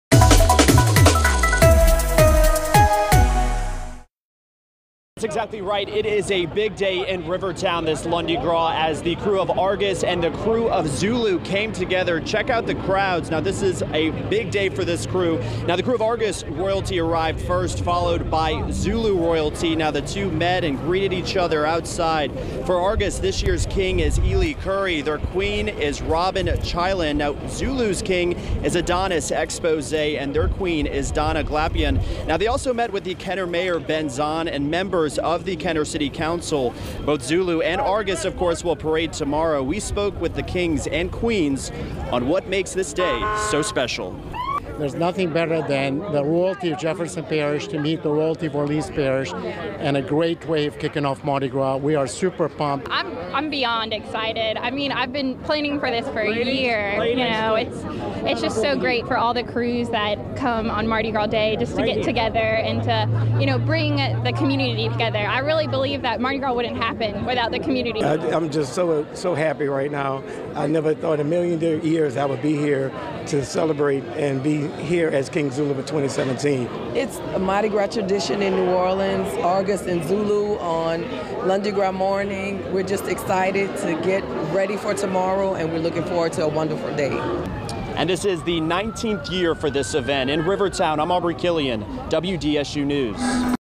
Argus and Zulu royalty join together for annual meeting in Rivertown
The royalty of Orleans Parish and the royalty of Jefferson Parish made their annual greeting on Lundi Gras in Rivertown.